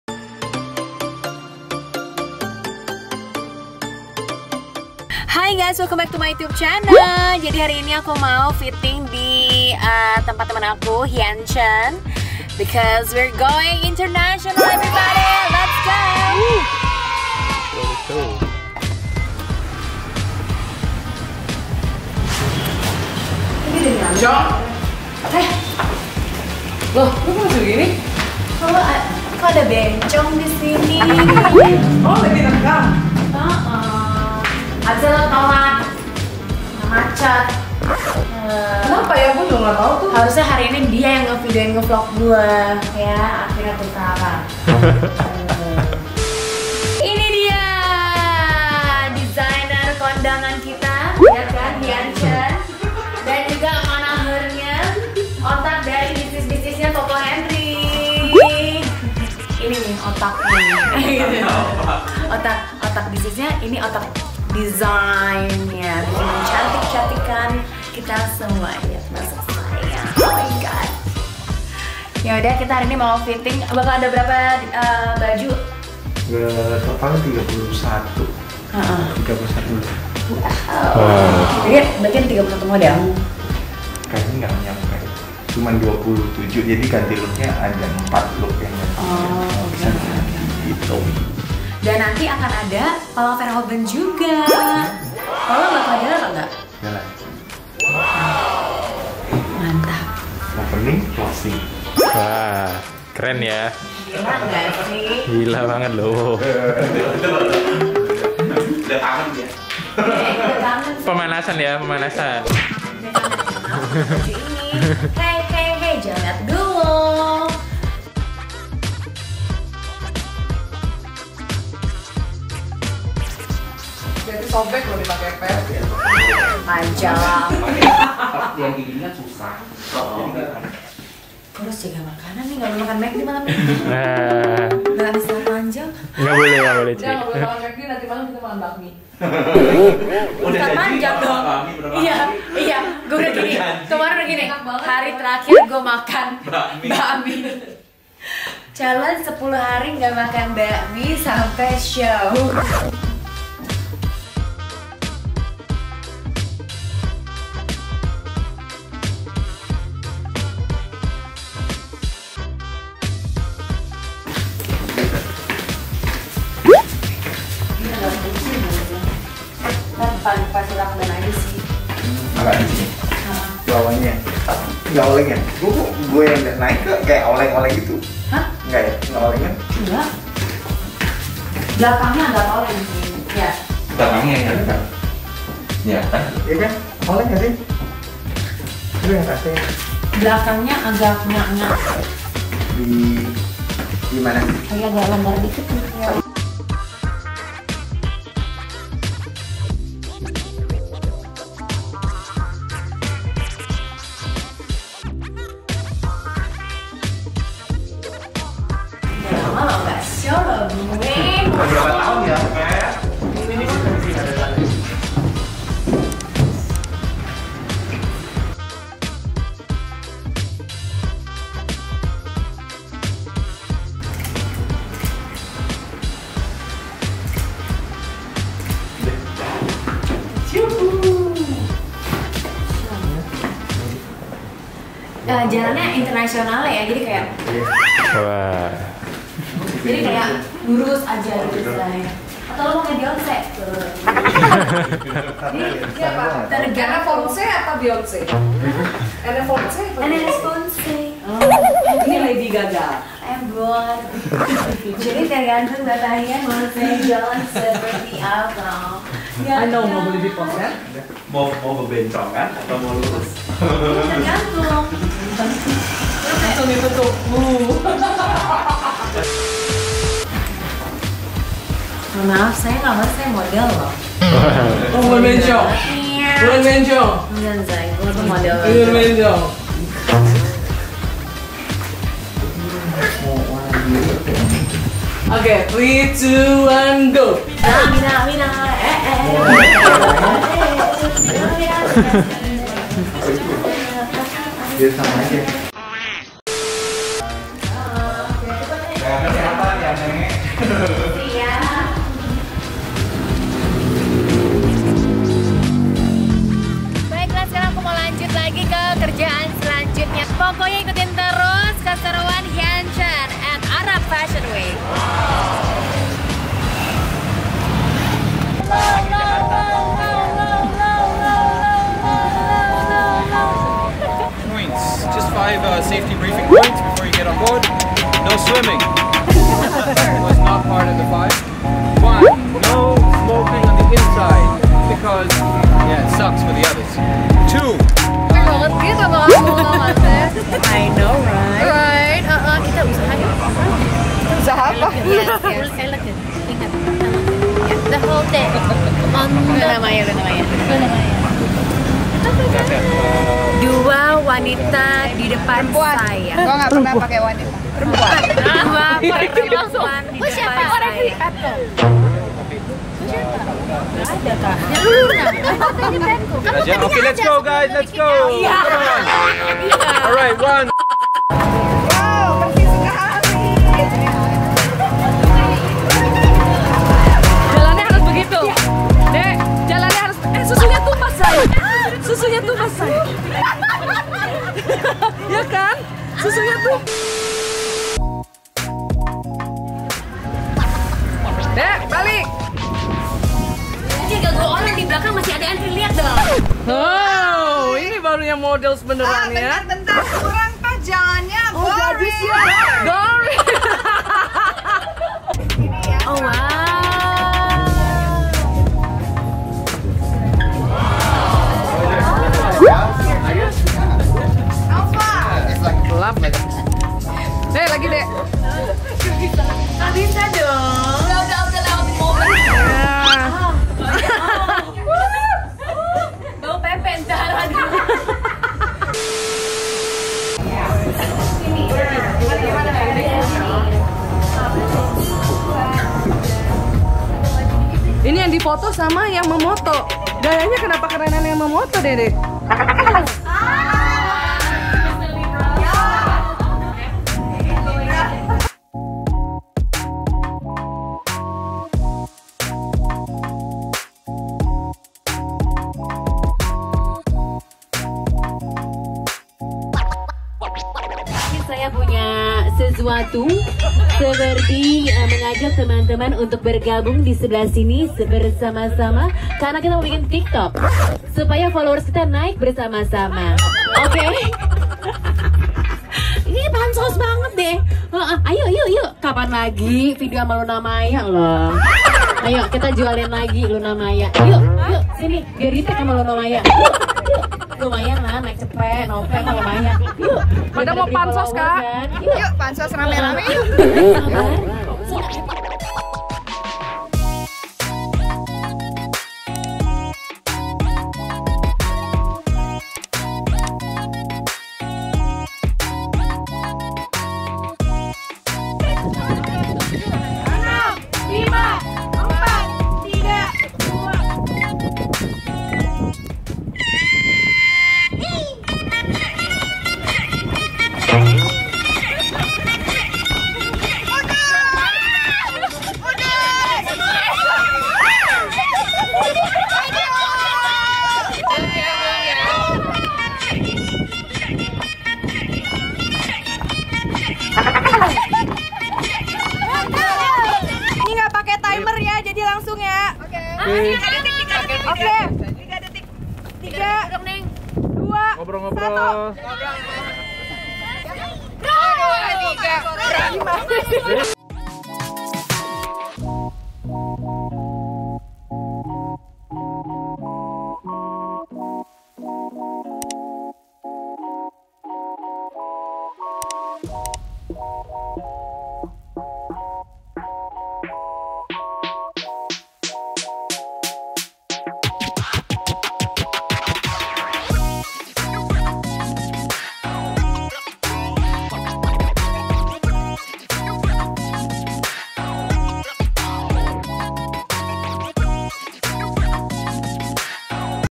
NAIK PINISI SAMBIL NONTON TRUNK SHOW MANDJAH DI TENGAH LAUT !!
1 minggu sebelum berangkat buat ke Arab Fashion Week eik mampir dulu buat fitting2 baju nya Hian dan sekaligus berangkat nonton in Trunk Show Ceunah di tengah laut gtu yah shayy..

[Indonesia]